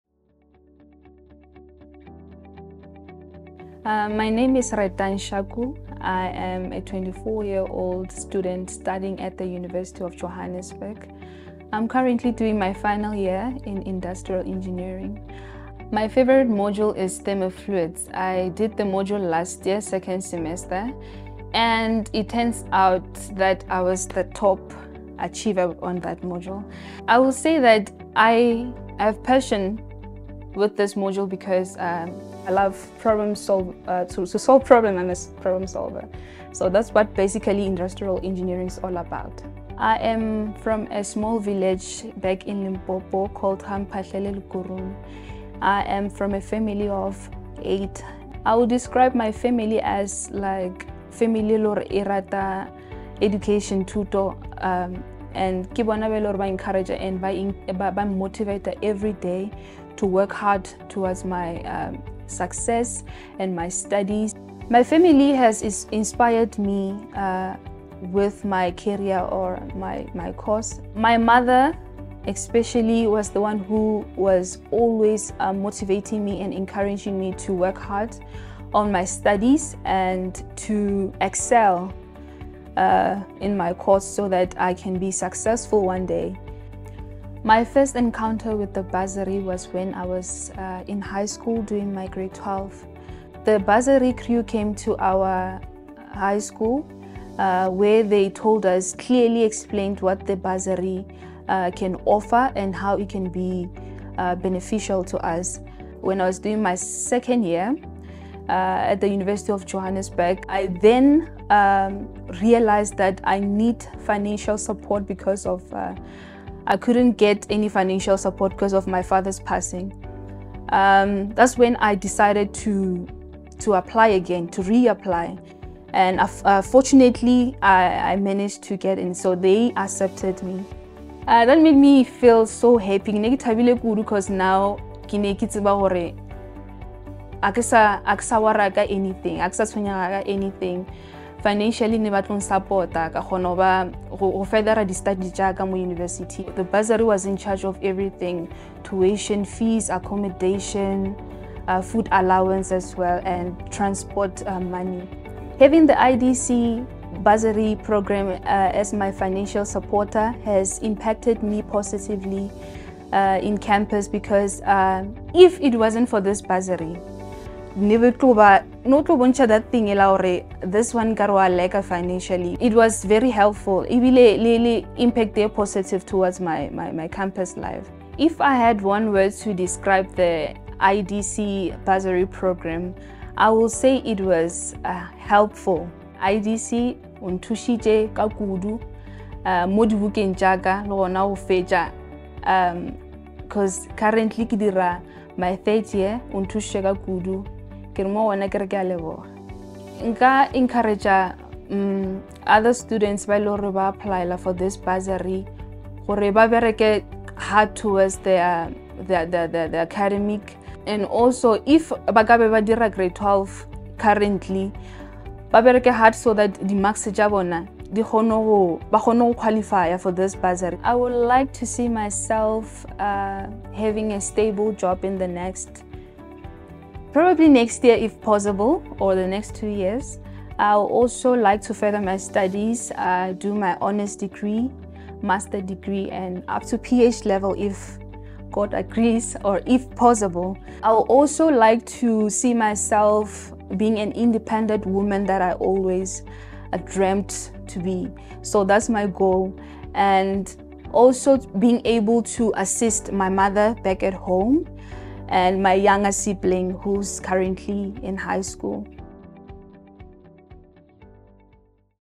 IDC Bursary Programme beneficiary- Retang Shaku
Retang Shaku, an Industrial Engineering student, shares how receiving a bursary from the IDC transformed her educational aspirations.